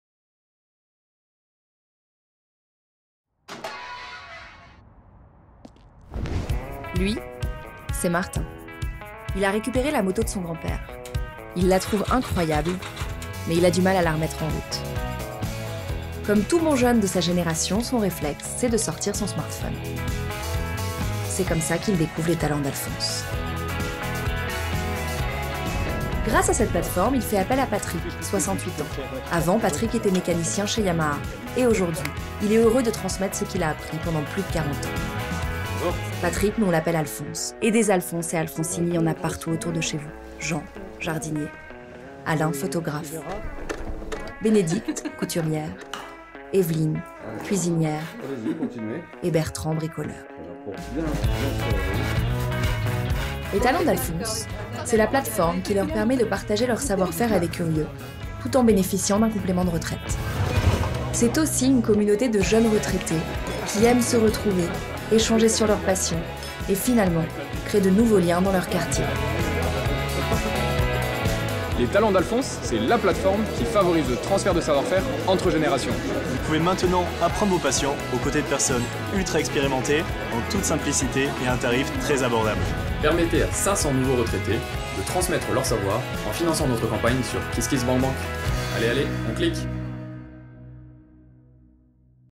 Les Talents d'Alphonse
Les Talents d'Alphonse est la plate-forme qui favorise la transmission de savoirs-faire manuels entre les générations.

Réalisation : Thomas Lehout
Cadrage : Thomas Lehout, François Xavier Pasquier, Laure Massiet
Son, mixage : Aurélien Thome
Montage : Marine Couderette, Thomas Lehout
Scripte : Alice Becue
Direction de production : Fanny Pelinq